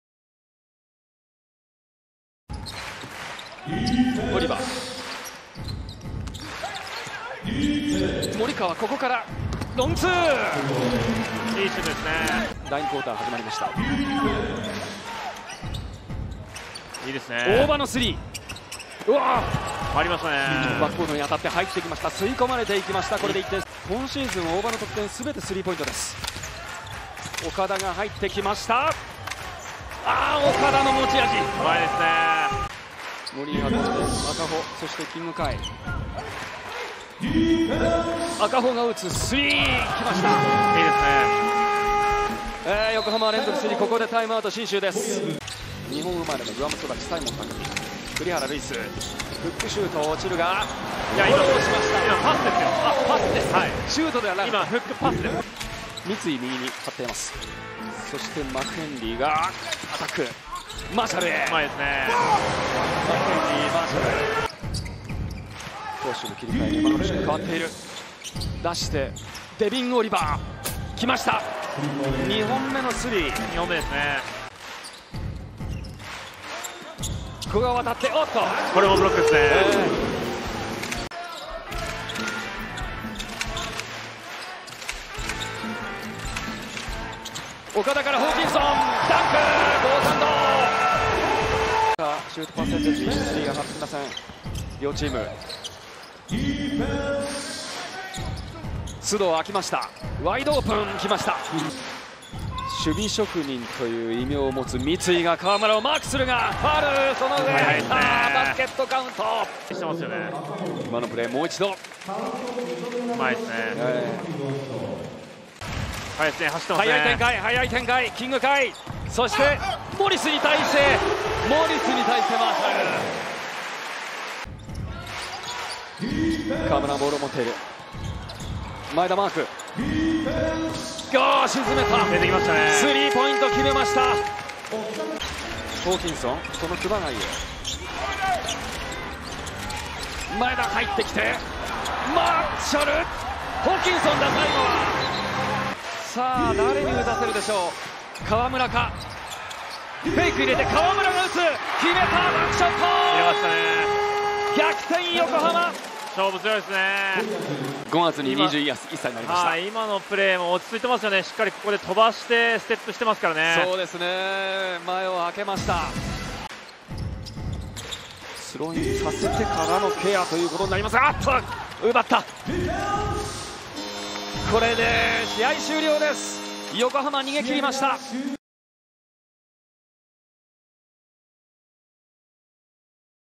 Shinshu Brave Warriors vs. Yokohama B-Corsairs - Game Highlights
Watch the Game Highlights from Shinshu Brave Warriors vs. Yokohama B-Corsairs, 10/26/2022

00:00:00 - 1st Quarter
00:00:12 - 2nd Quarter
00:01:04 - 3rd Quarter
00:01:55 - 4th Quarter